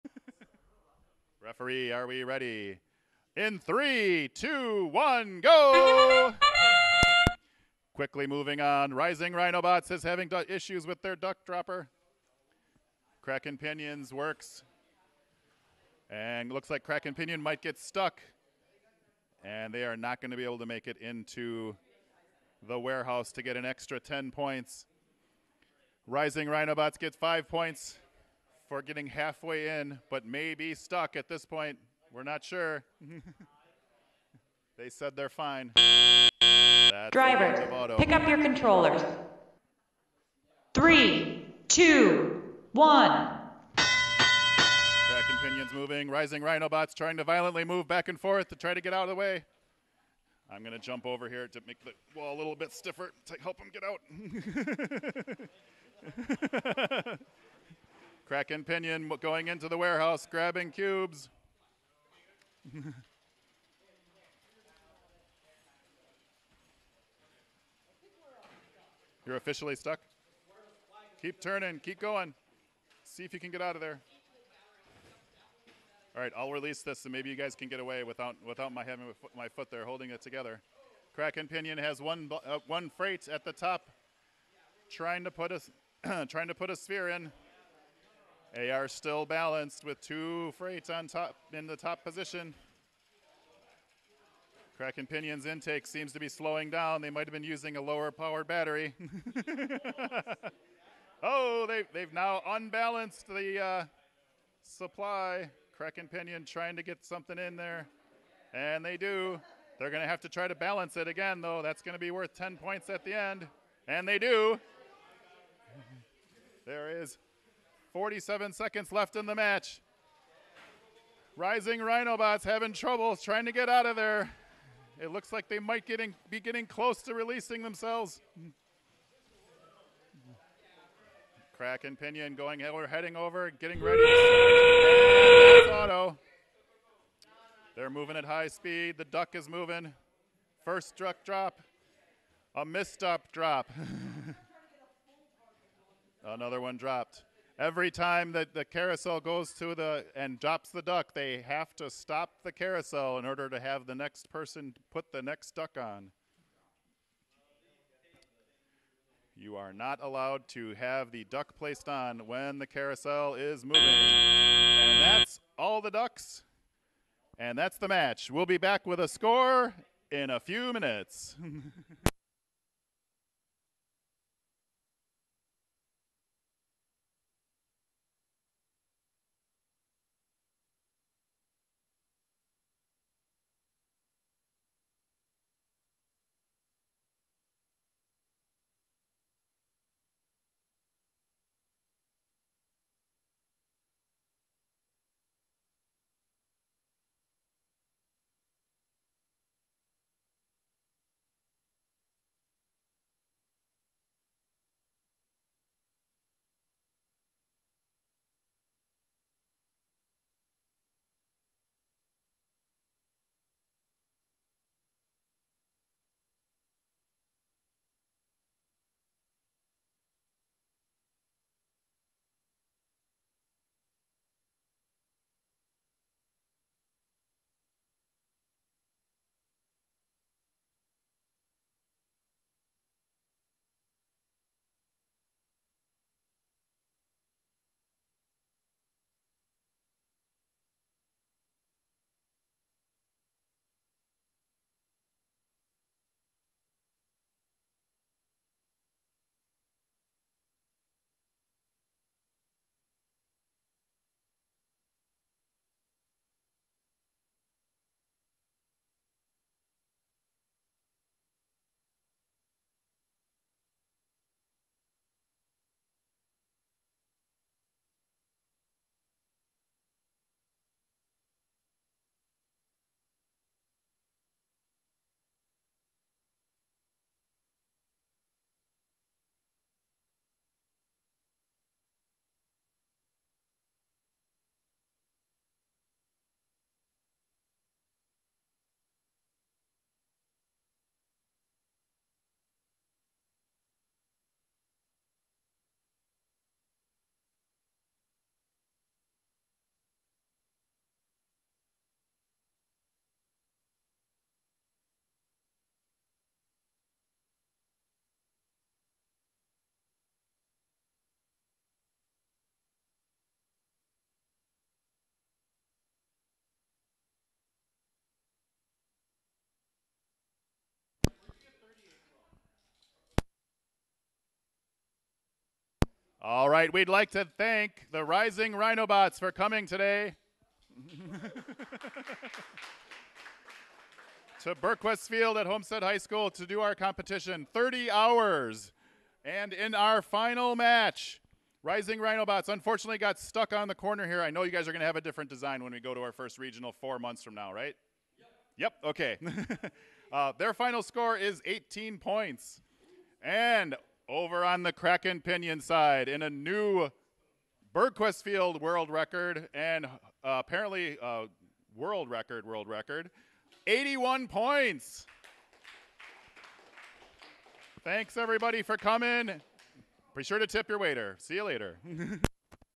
FTC Freight Frenzy World Record Score
Kraken Pinion gets 81 points in a match, being the current world record.